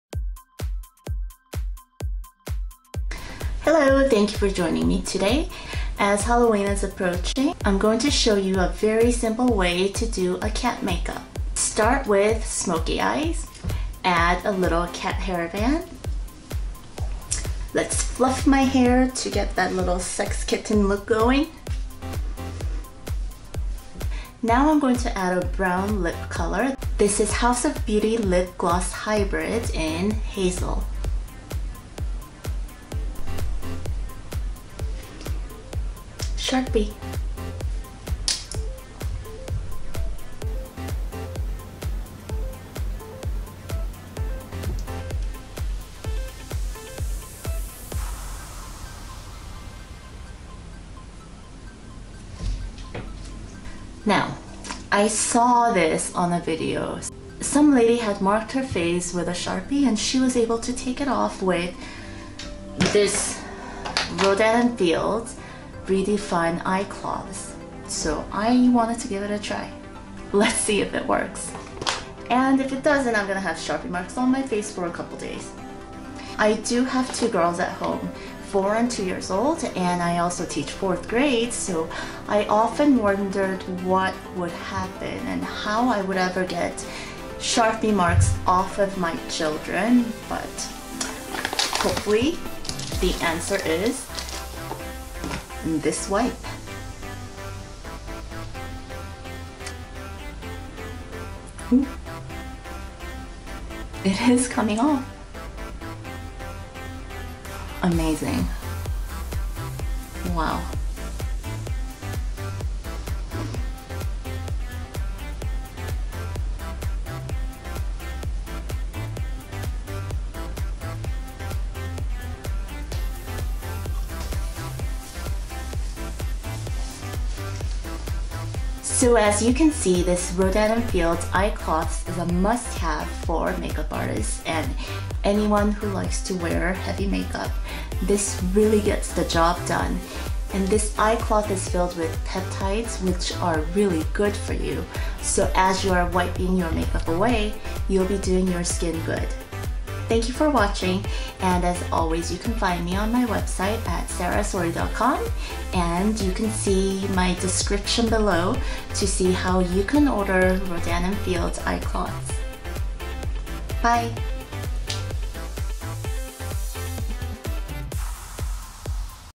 BEST MAKEUP REMOVER EVER! (It even erases Sharpie!)
Click the link above to shop and purchase Rodan + Fields Eye Cloths (Make-up Remover Sheets).

In this video, I do a quick cat makeup using a SHARPIE! I have never known any makeup remover that could take off Sharpie on skin.  However, Rodan + Fields Eye Cloths Sheets take off the darkest, thickest makeup as well as Sharpie marks.  Rodan + Fields Eye Cloths are packed with essential peptides so that while you are taking layers of makeup off of your face, you are actually applying beneficial peptides on your face.